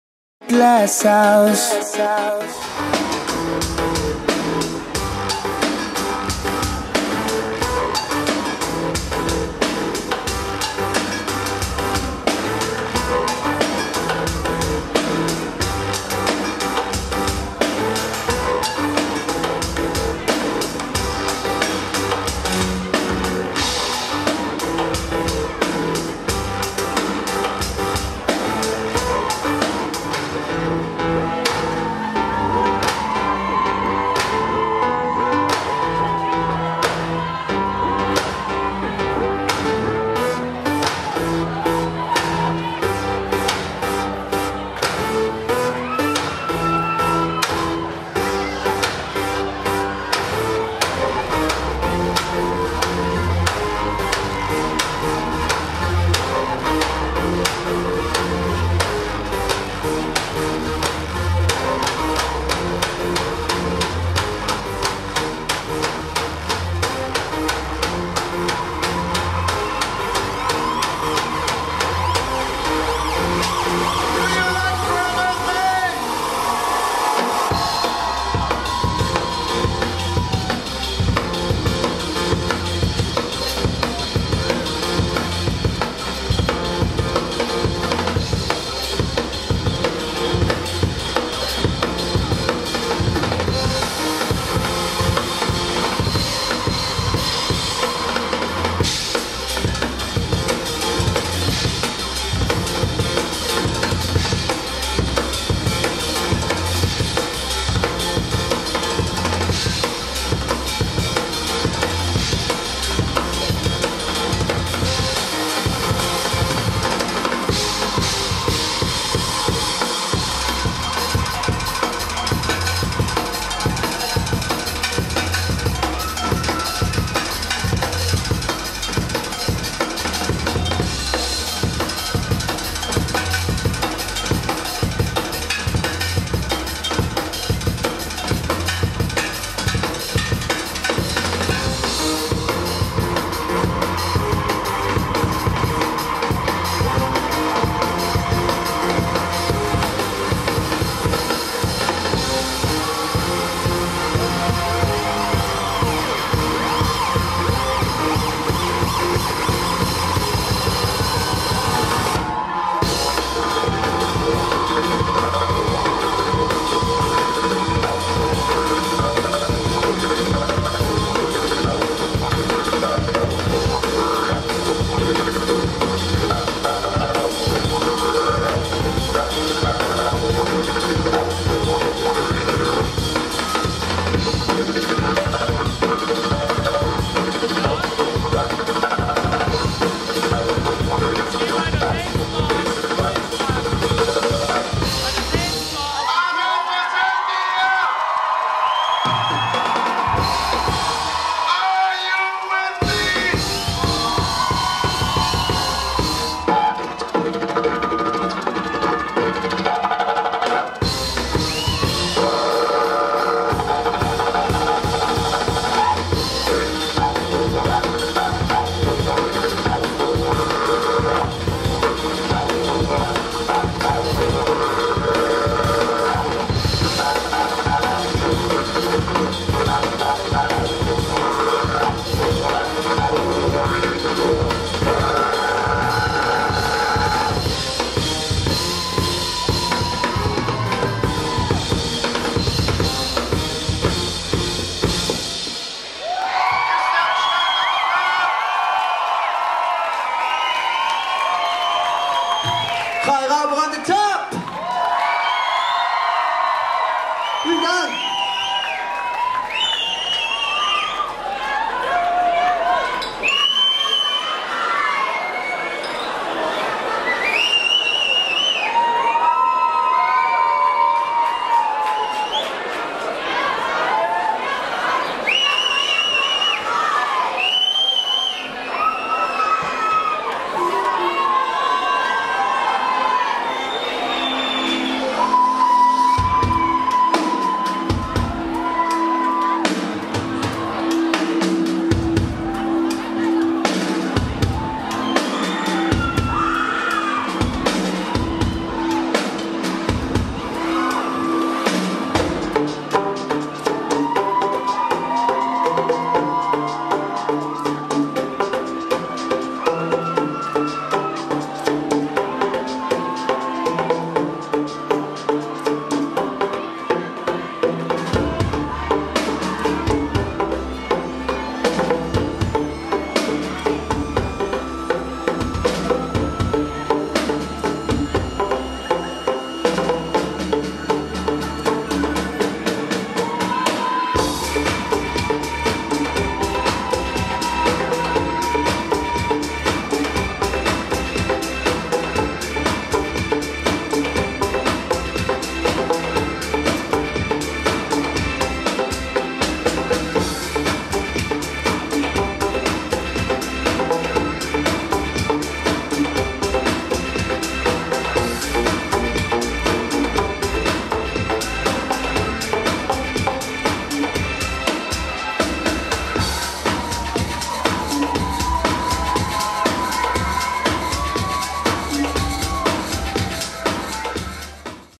Bubble Beatz LIVE in FullHD @ Glashaus Adorf 20.05.2011
5 Jahre Glashaus -- ein denkwürdiger Anlass mit Gratulanten der Superlative!

Denn niemand geringeres als die Megastars der RTL -- Show „Das Supertalent" BUBBLE BEATZ standen live auf der Bühne des Event -- Centers an der Waldbadstrasse. Bubble Beatz schafften es mit ihrer einmaligen Bühnenshow bis ins Finale der letzten Staffel und begeisterten nicht nur die Jury um Dieter Bohlen und Silvy van der Vaart sondern auch gleich noch ein Millionenpublikum.
Bubble Beatz als die „Mülltrommler" zu bezeichnen, mag einfach sein, ist jedoch nicht im Ansatz die Würdigung ihrer Bühnenperformance. Denn egal ob auf Open-Air Bühnen oder einem schweißtriefenden Club, Bubble Beatz sind überall zu Hause wo es um fette Beats und ein visuelles Spektakel geht. Die Intensität der beiden gelernten Drummer macht die Show zu einem echten Erlebnis, für das es keine Vergleiche gibt!